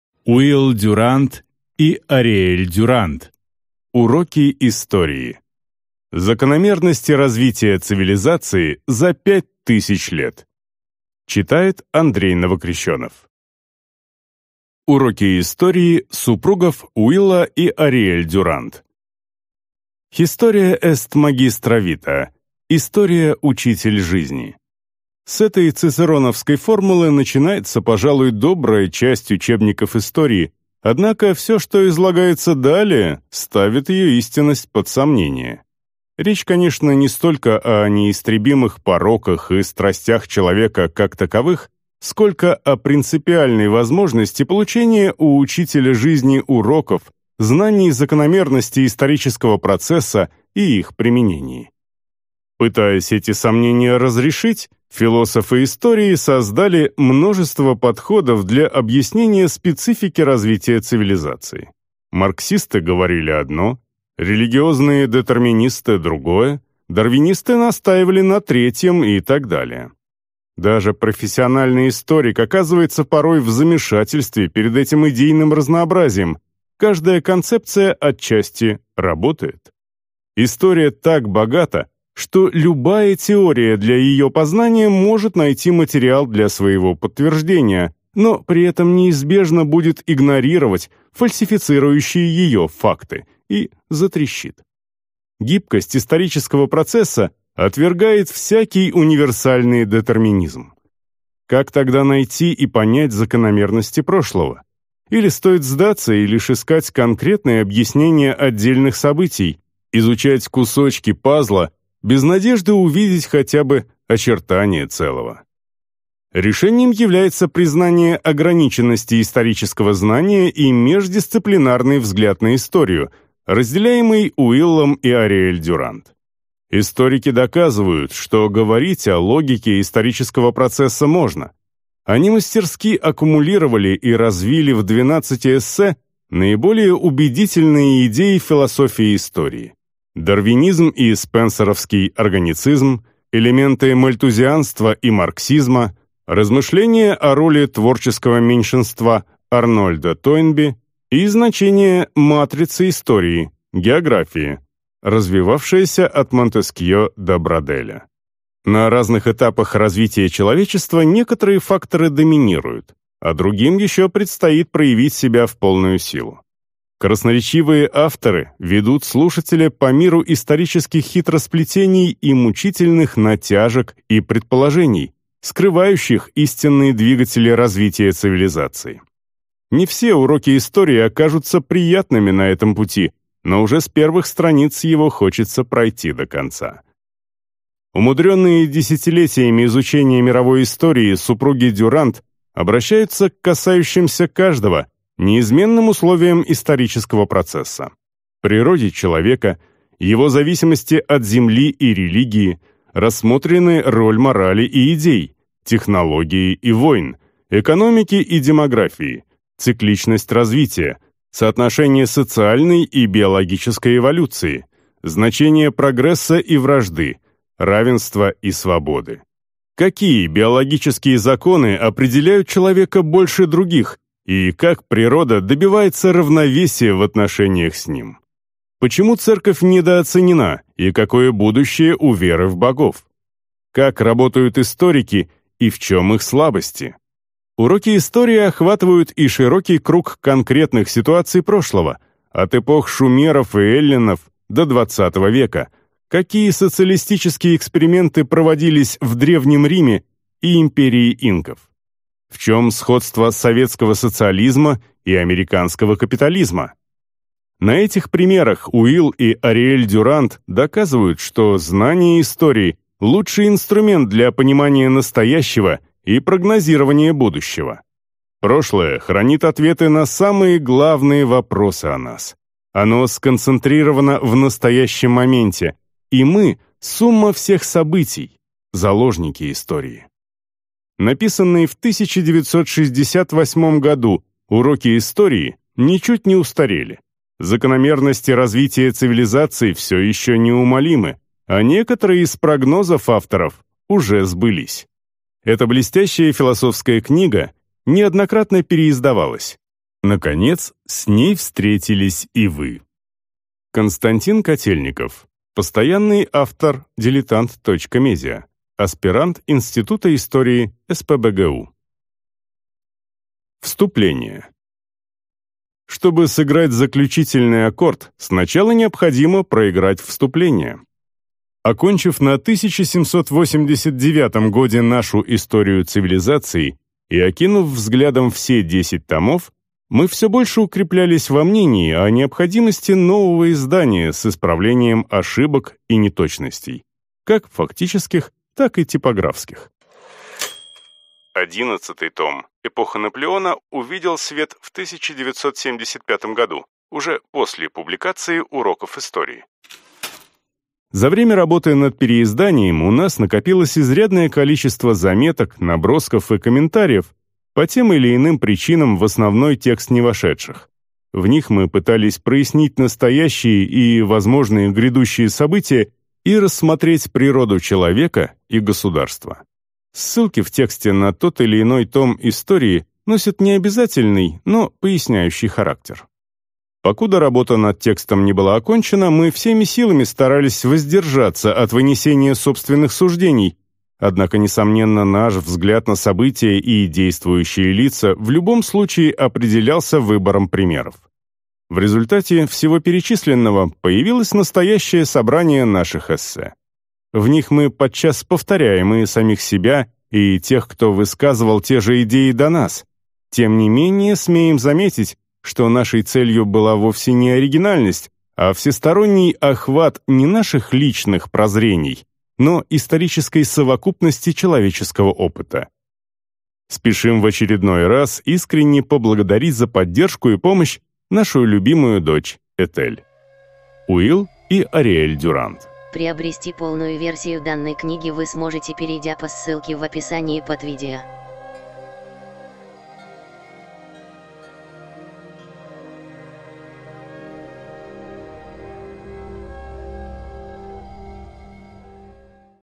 Уроки истории. Закономерности развития цивилизации за 5000 лет. Ариэль Дюрант, Уилл Дюрант
Ставшая классической книга об уроках истории от лауреатов Пулитцеровской премии и авторов исторического многотомника. Переиздается и успешно продается с 1968 года.

Авторы, профессиональные историки и академические ученые выявили закономерности общественного и исторического развития, цикличность исторического процесса и конечность любого социально-экономического строя. Основываясь на своих исследованиях и на философском подходе, они очень образно и увлекательно рассказали о том, какой путь прошла Западная цивилизация (от истоков до начала XIX века) и почему ее путь был сложен именно из этих вех.
На русском языке публикуется впервые.